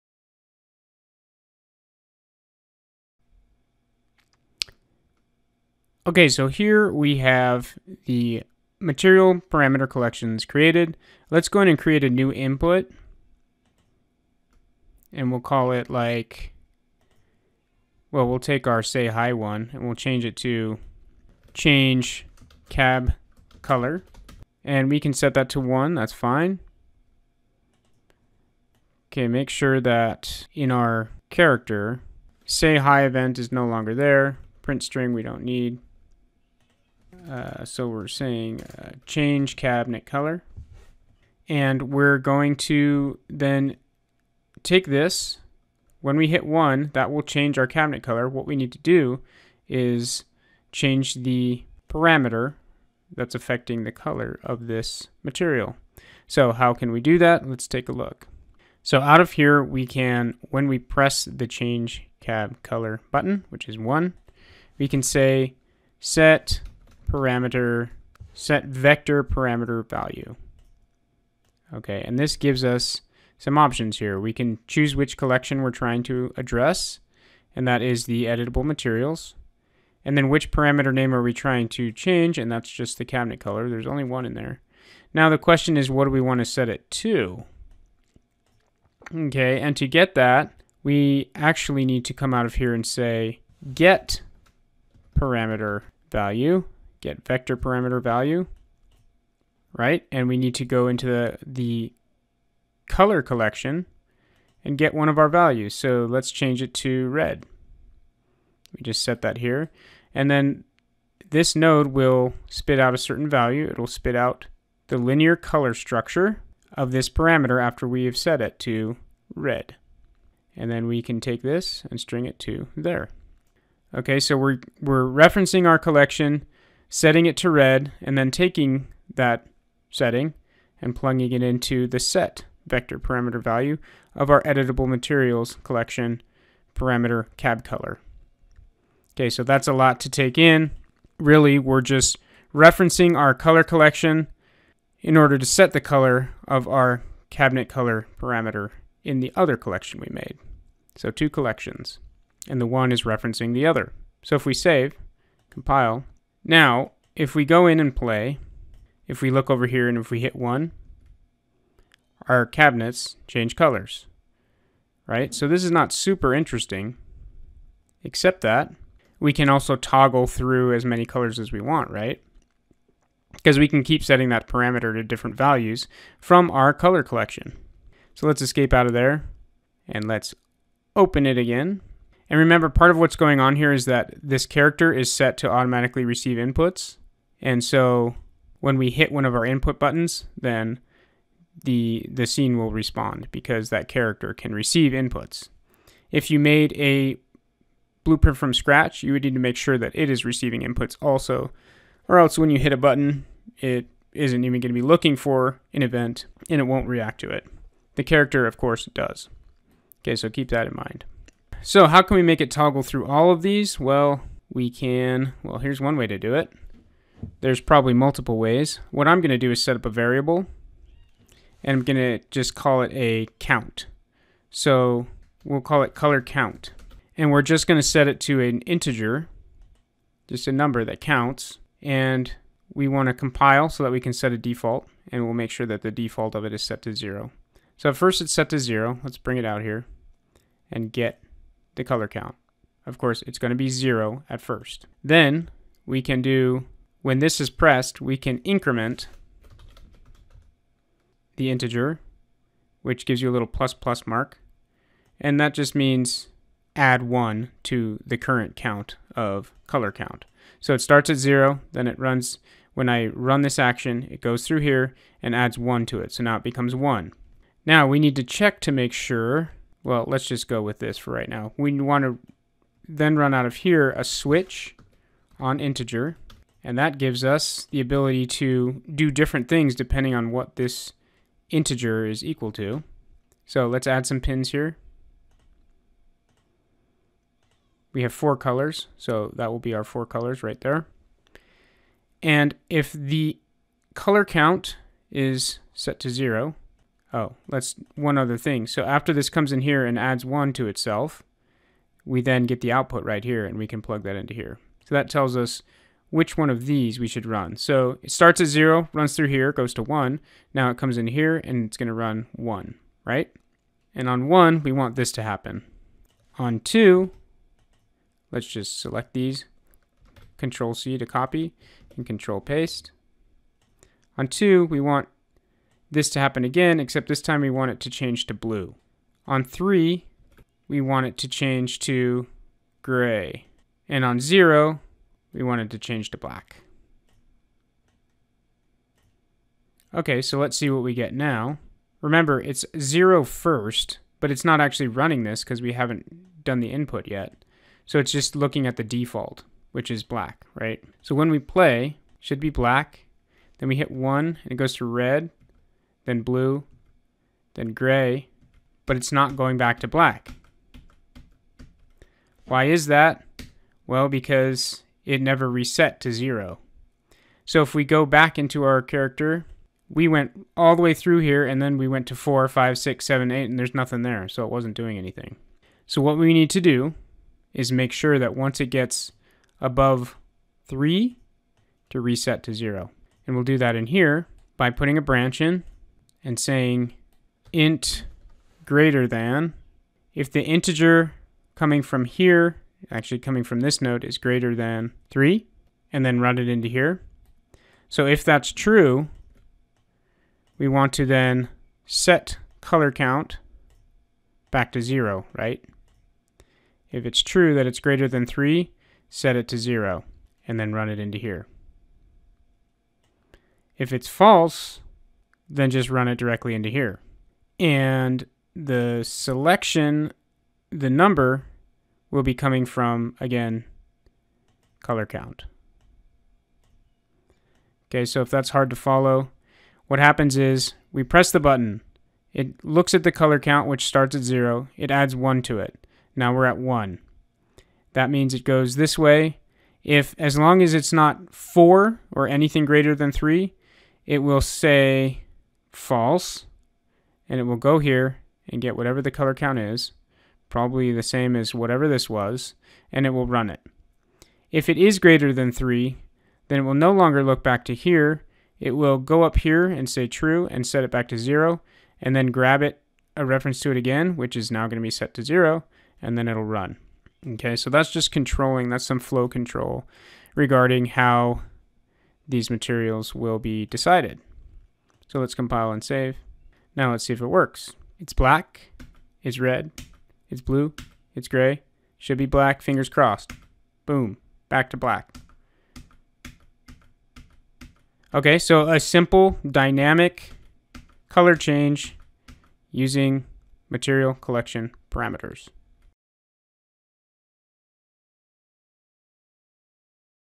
UE4 Tutorial: Changing Material Colors Dynamically With Blueprints and Parameter Collections
In this video I discuss one way that you can change material colors dynamically during game play in ue4. This technique comes in handy for architectural visualization (arch viz) projects where you want to interactively change the look and feel of a room or building.

This technique makes use of material parameter collections. I set up a material parameter collection full of color values, and one parameter collection with parameter called cab color. This second parameter is also plugged into the material I am trying to change as its base color. Because the material base color is set to equal that parameter in my collection, changing the value of the parameter changes the color of the material. The video will explain from there.

This is an easier way to adjust materials in real time (if all you need to change is the color) than setting up an array of different materials to toggle through.

The technique in the video also makes use of a custom input I set up. It is just an input called changeCabColor and it is mapped to be triggered when I hit a button on my keyboard.